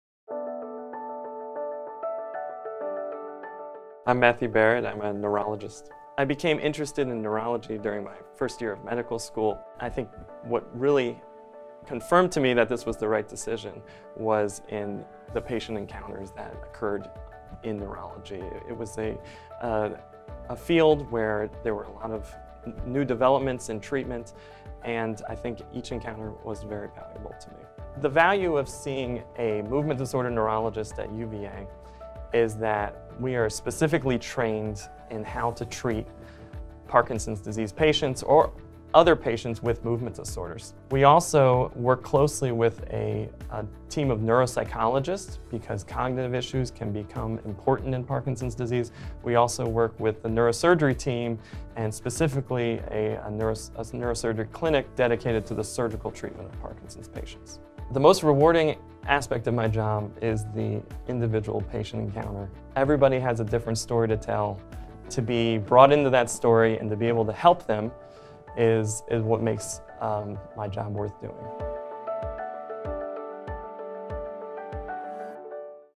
Meet UVA Neurologist, Dr. Matthew Barrett
Matthew Barrett, MD, is a neurologist at UVA Medical Center. Dr. Barrett specializes in movement disorders and sees patients who make struggle with Parkinson's Disease and Tourette Syndrome, among other movement disorders.